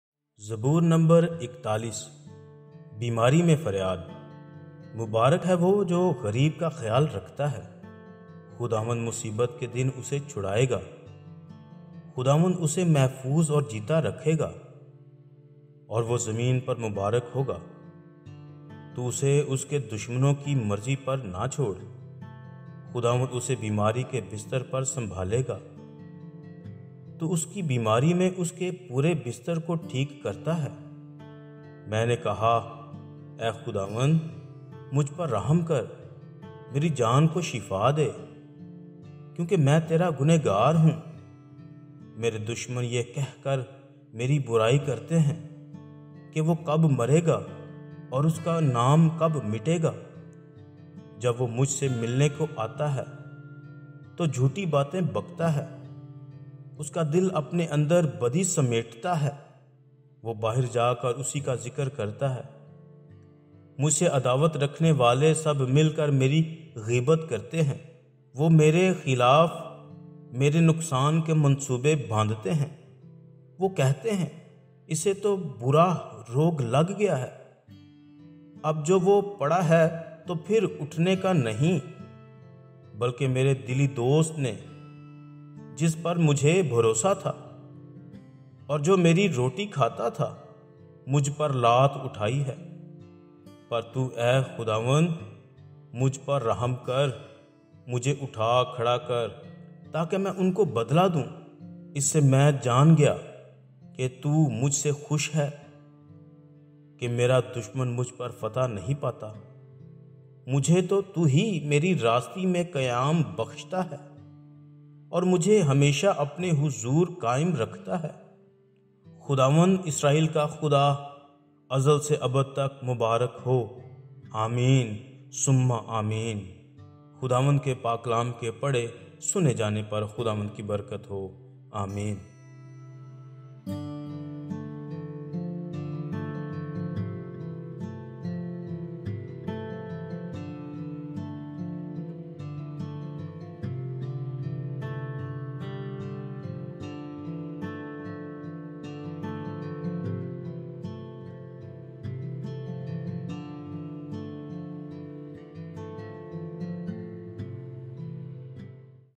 Zaboor 41 | Psalm 41 in Urdu | Urdu Bible Study || Bible Verses
Aaj ki Is video me maine Aapko Zaboor 41 ki talawat ki hai Ummeed Hai Aapko Ye Video Bahut Pasand Aayegi Thank you so much...